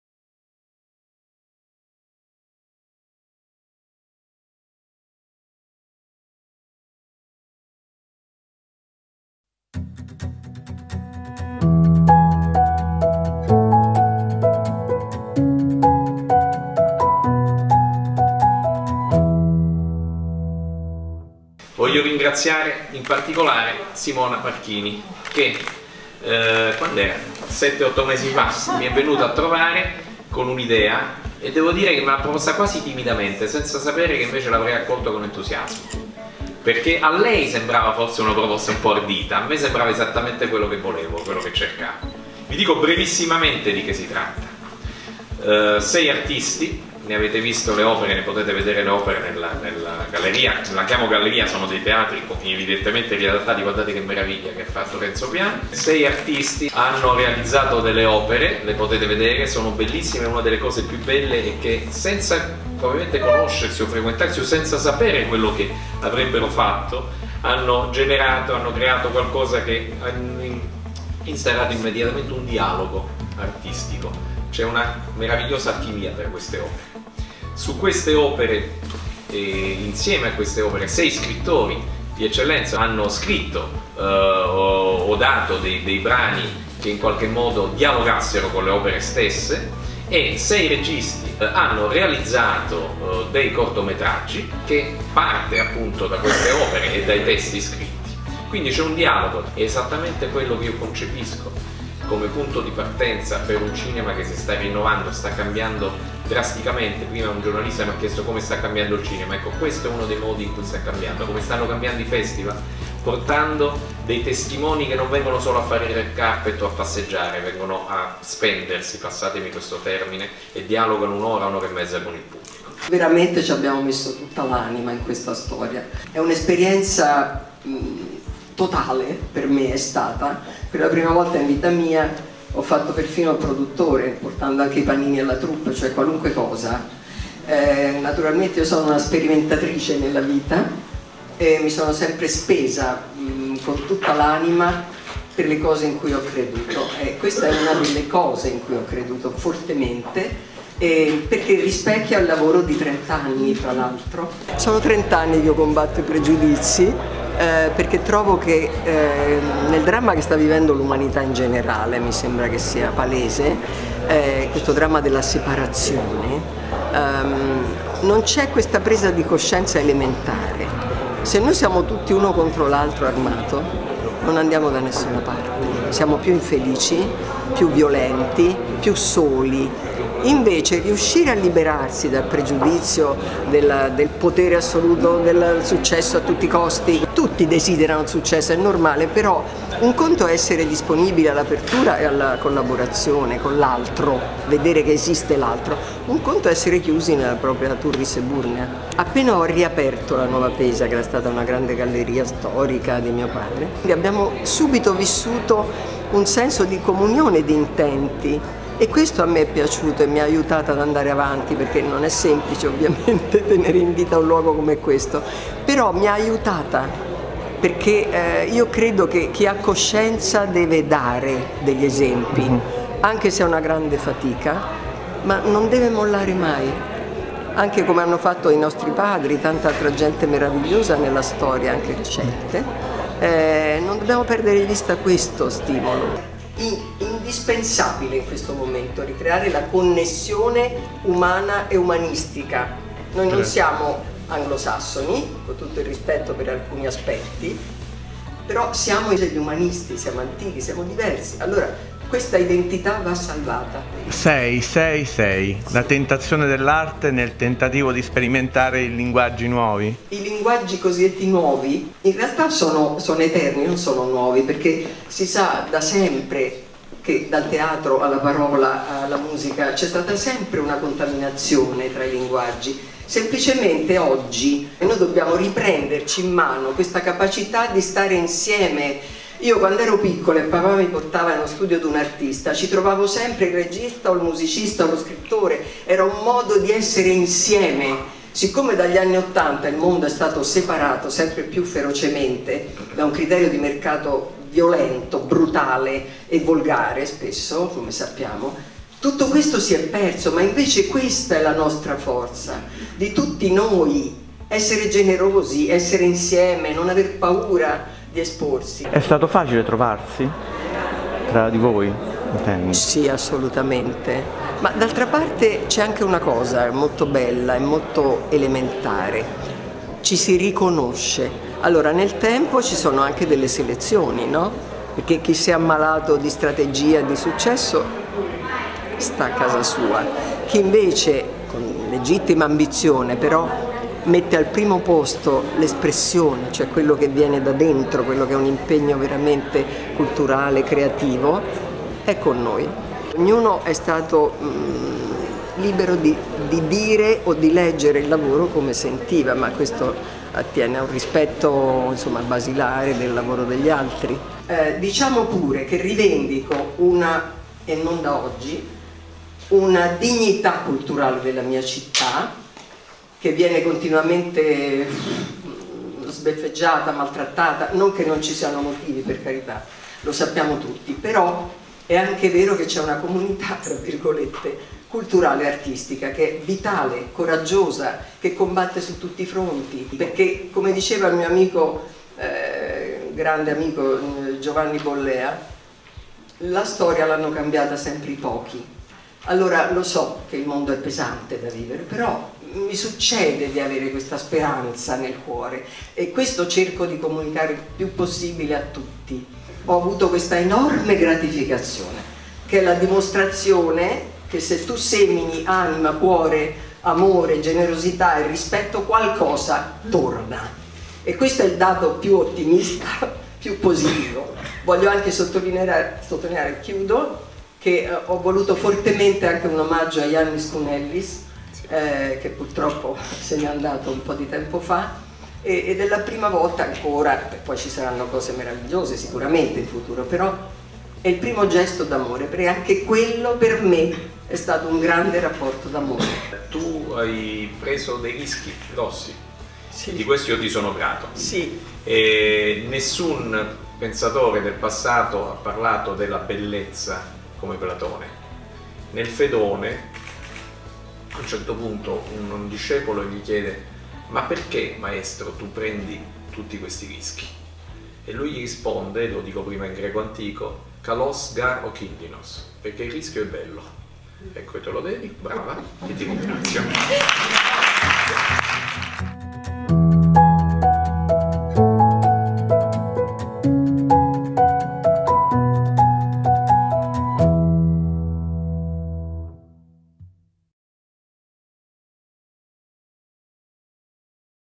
Festival del Cinema - Intervista a Simona Marchini per TRAME d'AUTORE
L’arte contemporanea diventa protagonista alla Festa del Cinema di Roma. All’Auditorium Parco della Musica, è allestita la mostra “Trame d’autore” un progetto promosso e ideato da Simona Marchini, in cui vengono presentate sei opere-video che nascono dall’interazione di sei artisti, sei scrittori e sei registi.

Le opere di Pizzi Cannella, Hidetoshi Nagasawa, Roberto Pietrosanti, Marina Sagona, Giuseppe Salvatori e Jannis Kounellis (primo omaggio postumo al grande artista) dialogano con la scrittura di Emanuele Trevi, Claudio Damiani, Jhumpa Lahiri, Marco Lodoli, Aurelio Picca ed Elena Stancanelli.

La narrazione che ne emerge si fa immagine attraverso sei cortometraggi che saranno proiettati negli spazi dello Studio 2 e 3, dove inoltre saranno riunite le opere già utilizzate come elementi drammaturgici.

Nella sua duplice veste di attrice e gallerista, Simona Marchini ha voluto mettere in comunicazione il linguaggio delle arti visive e quello della letteratura per dare forma a un linguaggio terzo e fusionale: quello del video d’autore. Le foto del backstage, realizzate da Dino Ignani, saranno proposte a La Nuova Pesa (via del Corso 530, Roma) per tutto il periodo della Festa del Cinema.